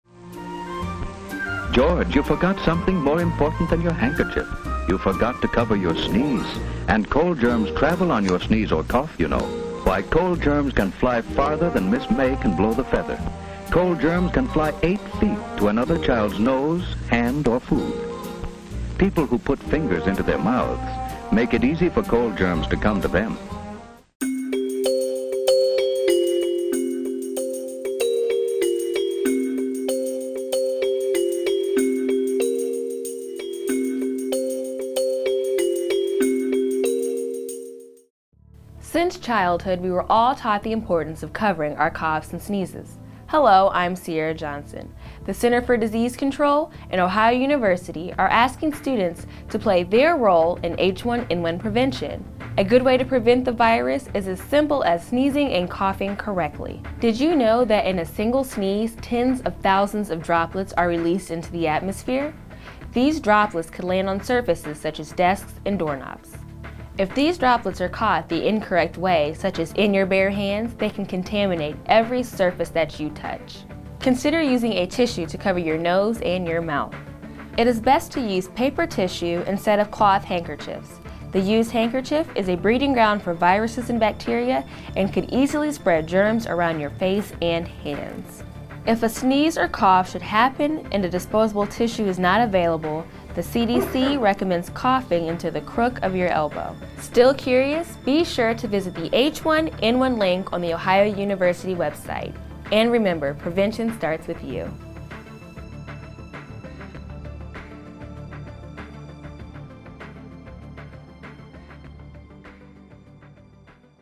Flu Facts: How to Handle Coughs and Sneezes
According to White House Advisors, the H1N1 virus may hospitalize 1.8 million patients in the U.S this year. This video offers tips that may increase your chances of staying healthy during a potentially deadly flu outbreak.